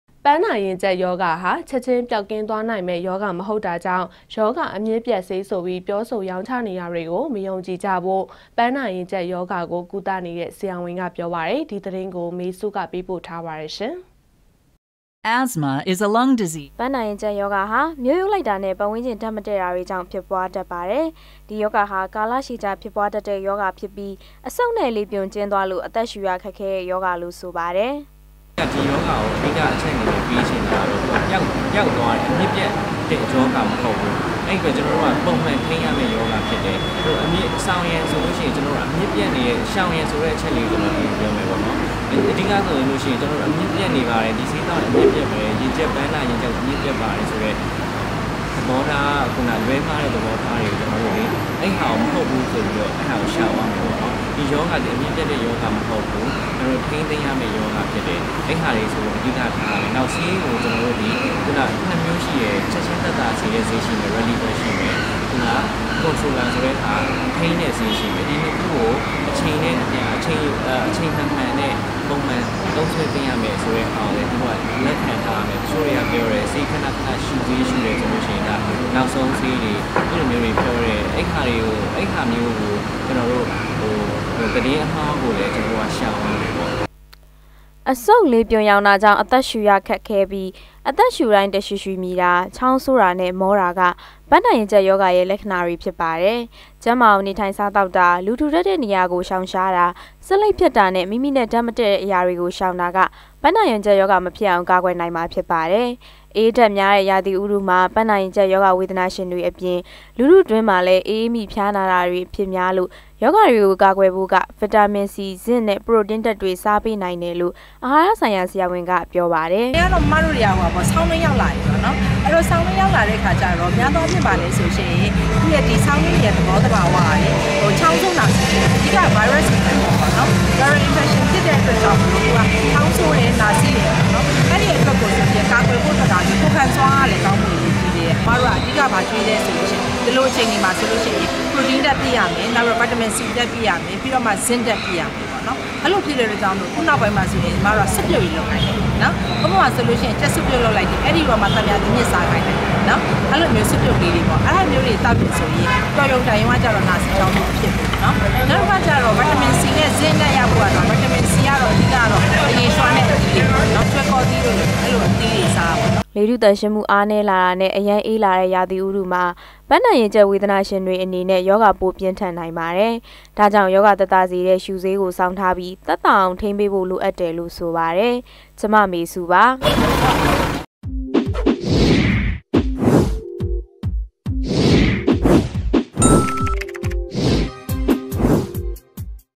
DVB - ပန္းနာရင္ၾကပ္ေရာဂါ အျမစ္ျပတ္ေဆးဆိုၿပီး ေရာင္းခ်တာေတြကို မယုံၾကည္ဖို႔တိုက္တြန္း
ပန်းနာရင်ကြပ်ရောဂါဟာ ချက်ခြင်းပျောက်ကင်းသွား နိုင်မယ့်ရောဂါမဟုတ်တာကြောင့် ရောဂါအမြစ်ပြတ်ဆေးဆိုပြီးပြောဆိုရောင်းချတာတွေကို မယုံကြည်ဖို့ ပန်းနာရင်ကြပ်ရောဂါကို ကုသနေတဲ့ ဆရာဝန်ကပြောပါတယ်။ ဒီသတင်း ကိုမေစုပေးပို့ထားပါတယ်။